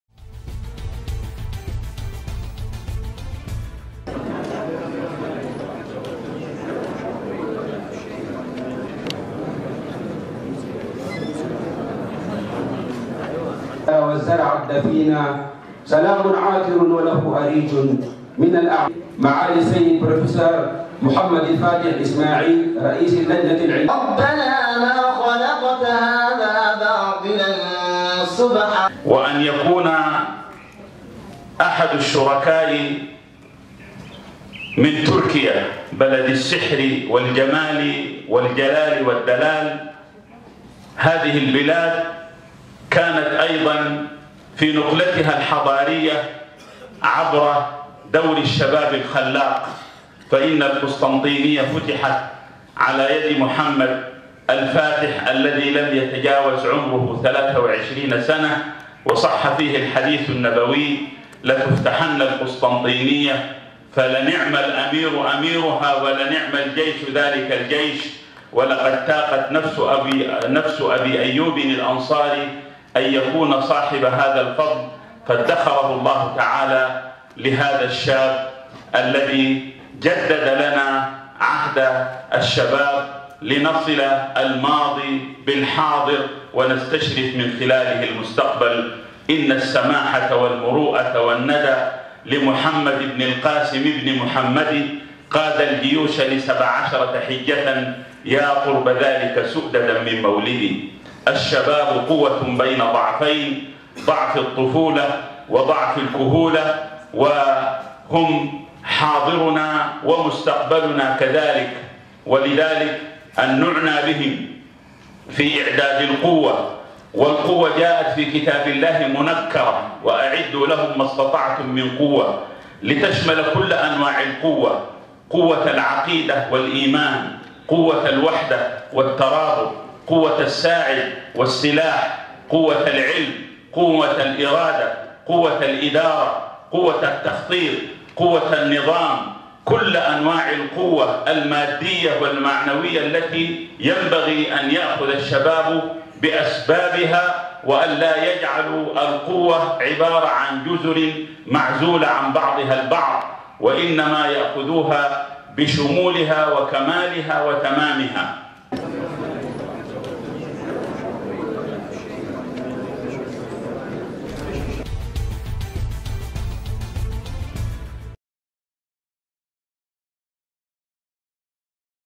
مصر العربية | انطلاق أعمال مؤتمر قمة "الشباب المسلم" في الخرطوم
انطلقت أعمال مؤتمر قمة "الشباب المسلم" بالعاصمة السودانية الخرطوم، بمشاركة 150 شخصا يمثلون 120 دولة.